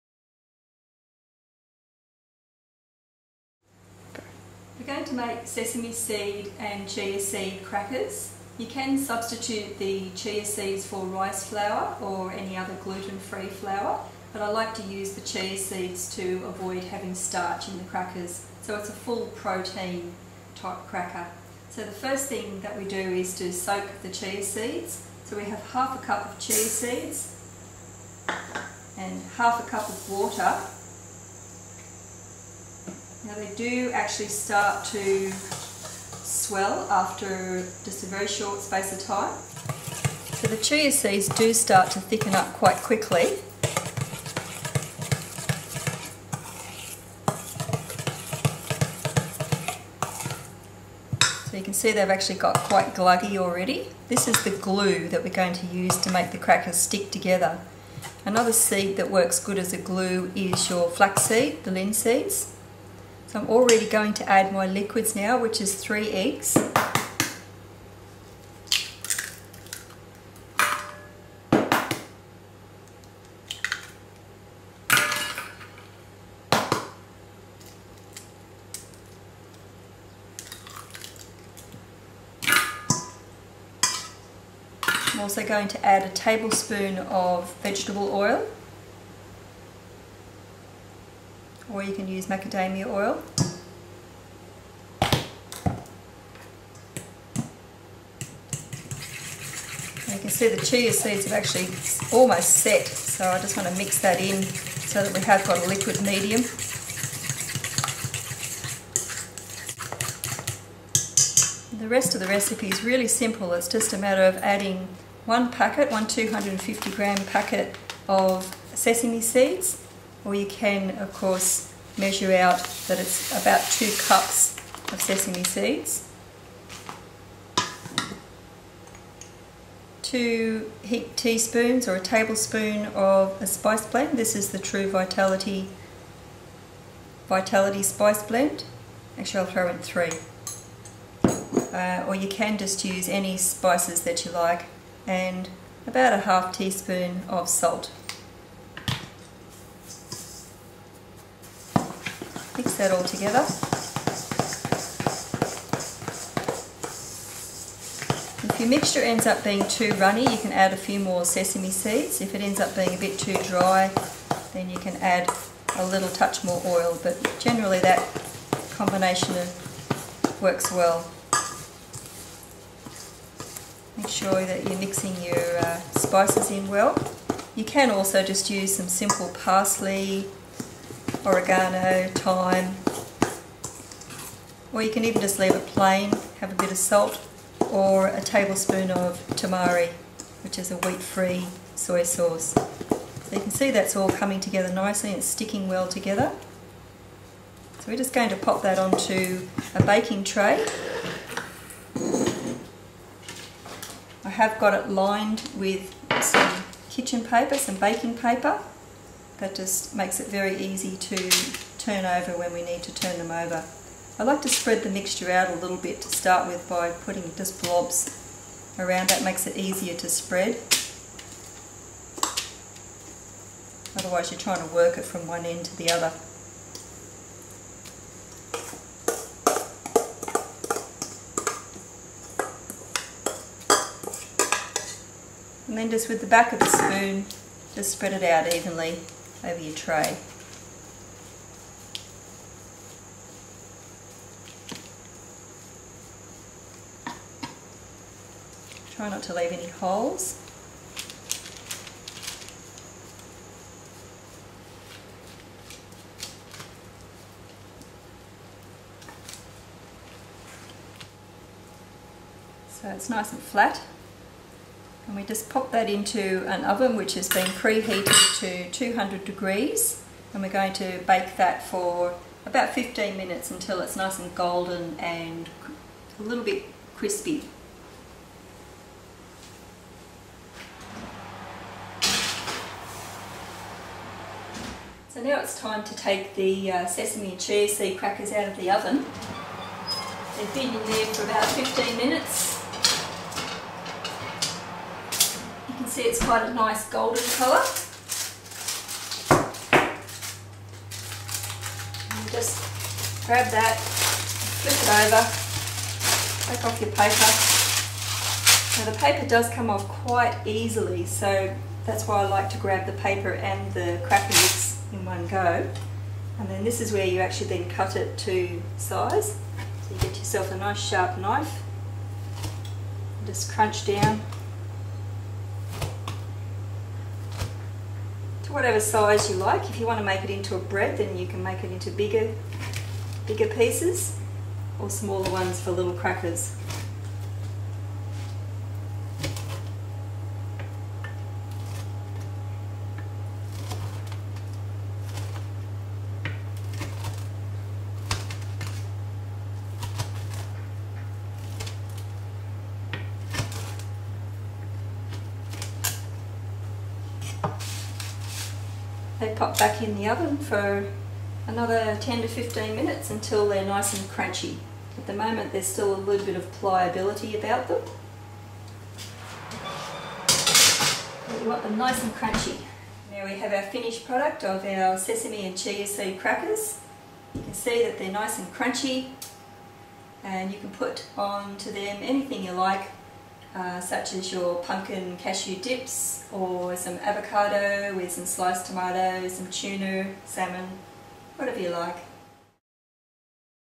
Sesame and Chia Seed Crackers
These tasty morsels are a full protein cracker so it won't carb you. Is great with tuna, avocardo, tomato etc or the pumpkin dip in the other recipe